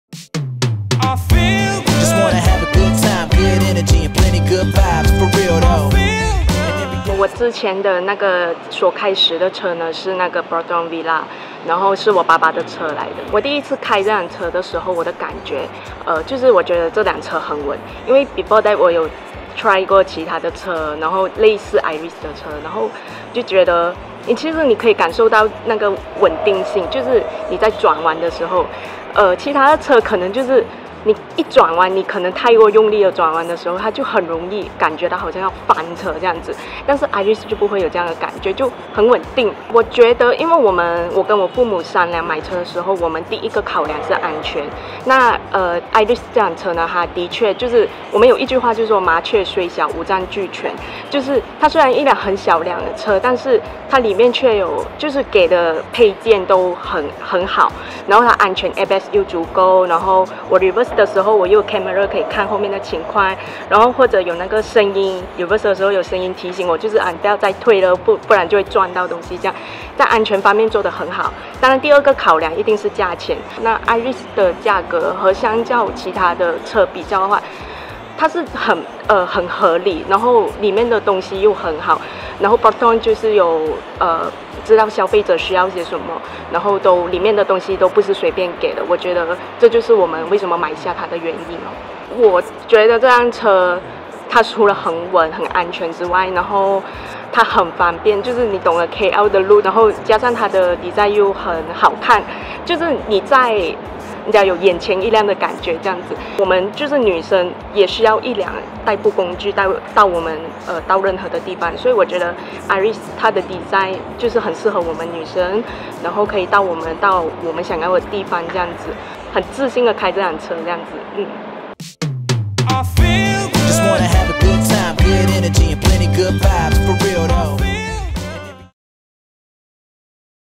2019 Iriz - The Sporty Intelligent Hatchback
A true Intelligent hatchback that stands out from the rest. The sporty outlook accentuated with distinctive features make the 2019 Iriz Ms. Kuan Chin Yee’s perfect choice for her everyday drive.

Good handling, safety features and most importantly, practicality for ease of driving on the busy streets of Kuala Lumpur are features that won her heart. Hear more about why the 2019 Iriz is more than a mode of transportation but a companion for Ms. Kuan to get through the day.